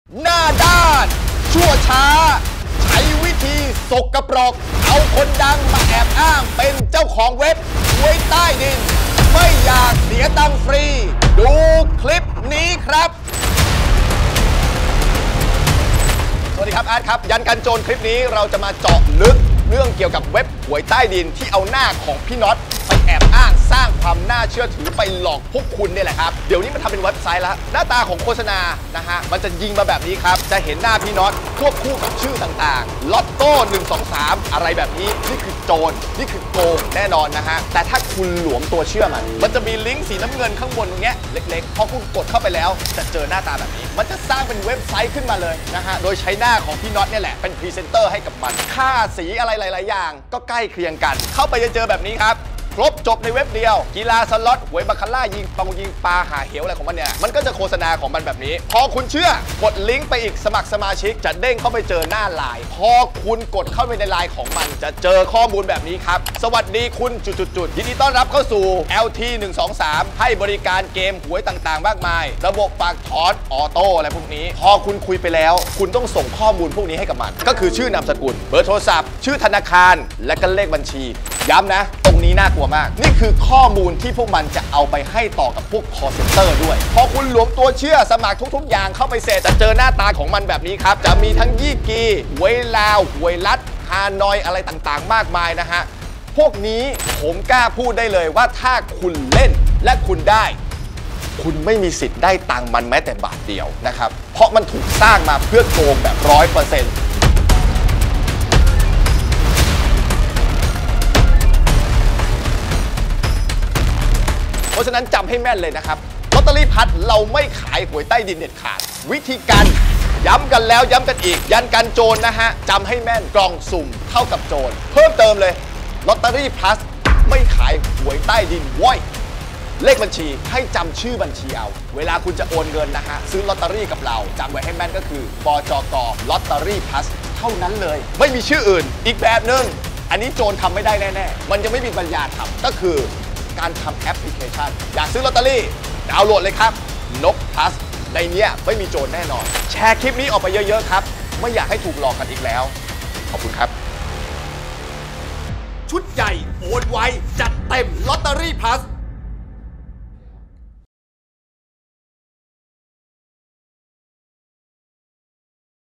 ลอตเตอรี่พลัสไม่เกี่ยวกับเว็บ LT123 !!!! l EP.3
LT 123 = เว็บพนันโกงเงิน

เราจะมาคุยกลโกงมิจฉาชีพ ที่มาในรูปแบบของการปลอมเป็นลอตเตอรี่พลัสแล้วหลอกคนให้คลิกสมัครไปเล่นเว็บพนันแล้วโกงเงิน ถึงเล่นถูกก็ไม่มีทางได้เงิน ถ้าไม่อยากเสียเงินต้องดู !

📌โหลดแอปนกพลัสได้เลยที่นี่